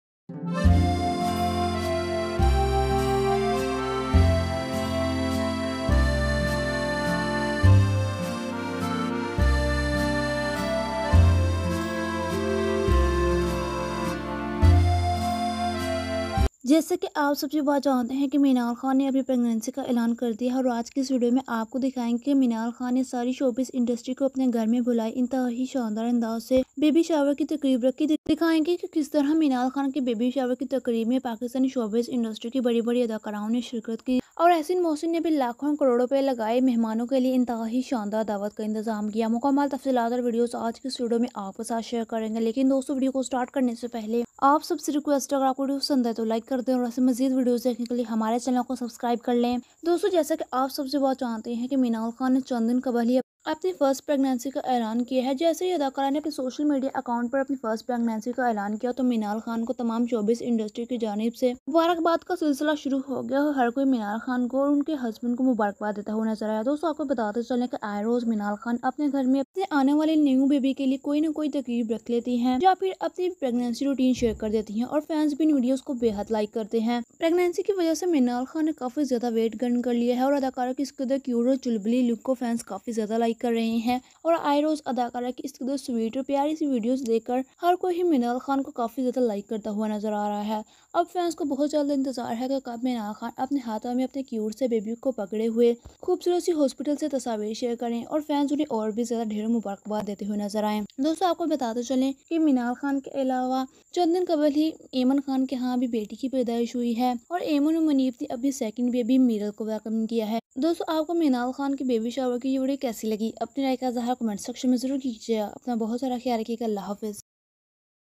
Minal khan baby shower, Aiman kham reveals Miral face
anzela abbasi,

javeria abbasi,

naimal khawar,

aliza sultan,

madiha imam marriage , aiman khan 2nd baby delivery,